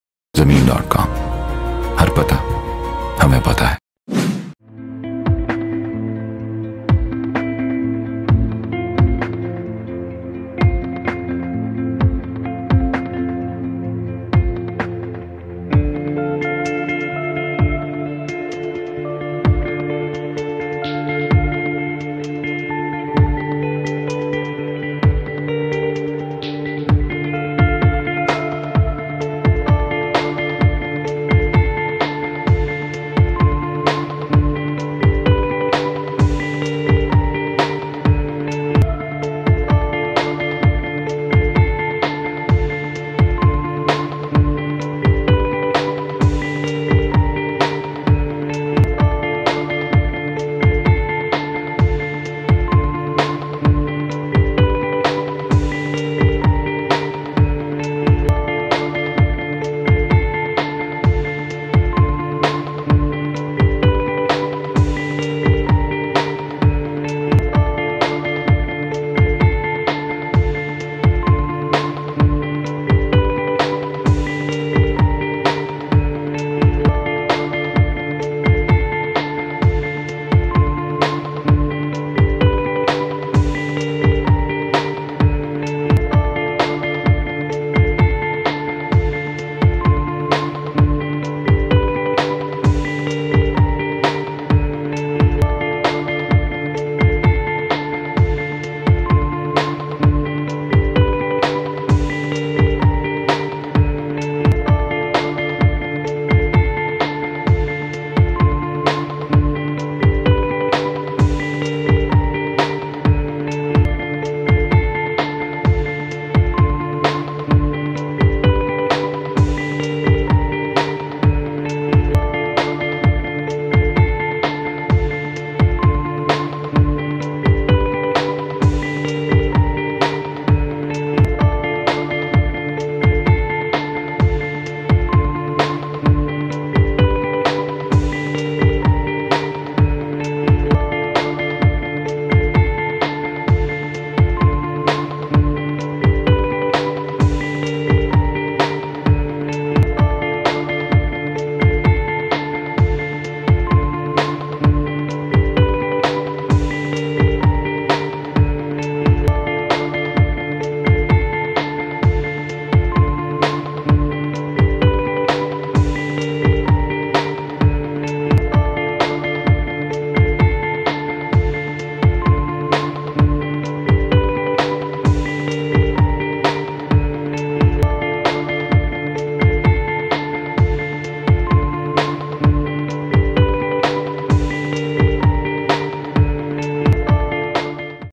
8 MARLA RESIDENTIAL PLOT FOR SALE IN BLOCK F B-17 MPCHS - MULTI GARDENS ISLAMABAD
Residential Plot is available for sale in B-17 with measurement of 1800 Sq. ft. . Residential Plot is located in B-17 which is surely the safest and finest societies in Islamabad. Buying this property will certainly give you comfort and peace of mind.